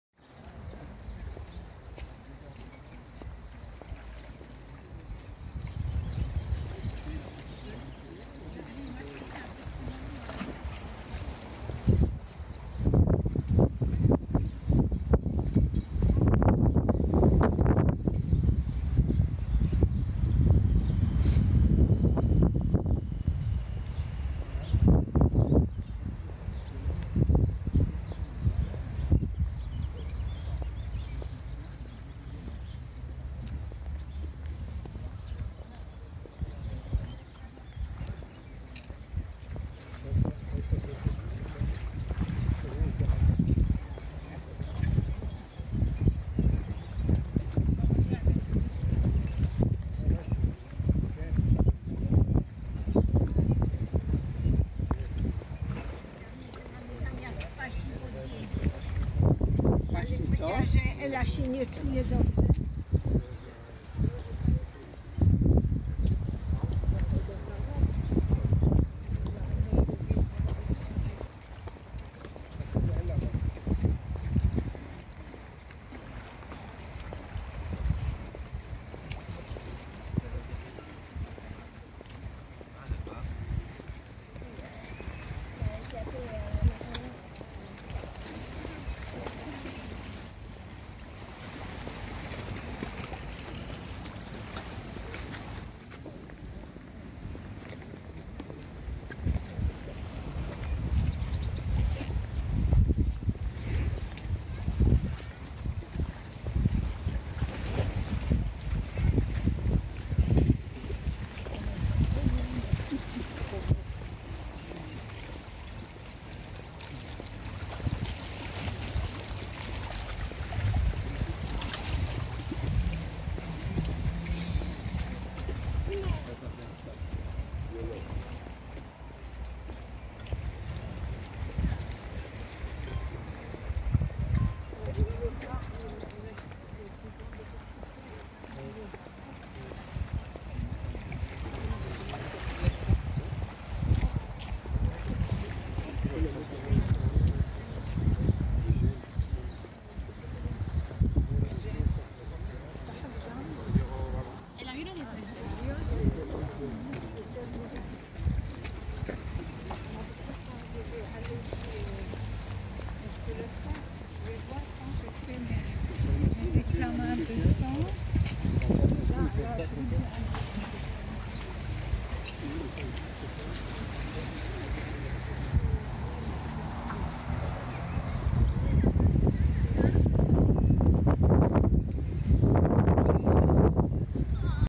Suisse. Switzerland. Morges. Швейцария. Фестиваль тюльпанов. Набережная.
Почти всю набережную можно увидеть в видео.
Погода не солнечная. Но вид гор и озера впечатляет.
Продолжение следует...